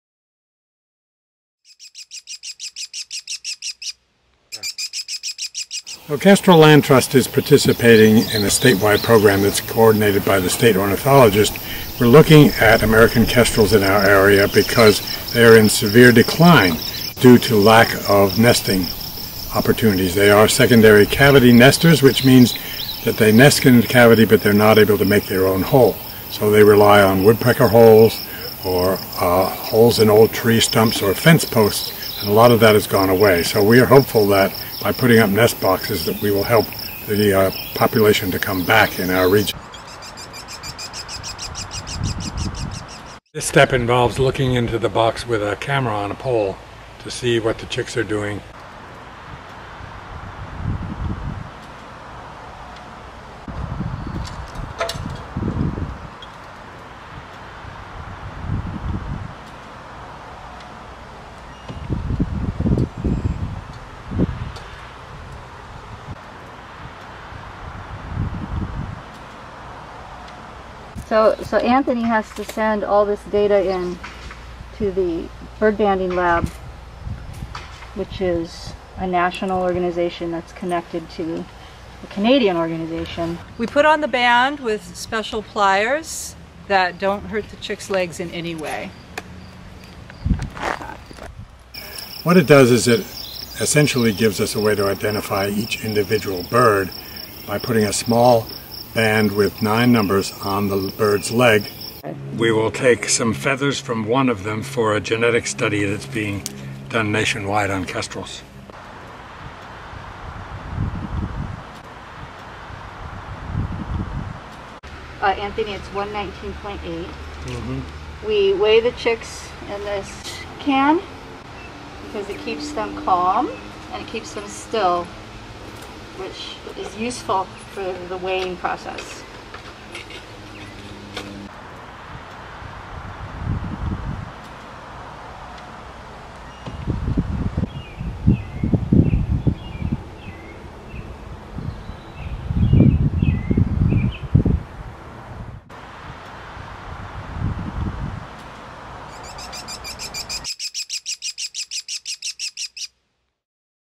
Kestrel Nest Box Project: Banding Chicks at Quonquont Farm
American Kestrels have been declining in the Pioneer Valley because their habitat has been lost to development. The Kestrel Nest Box Project provides breeding sites where we can collect important data about each chick. See how the process works with Kestrel Land Trust board and staff at Quonquont Farm in Whately.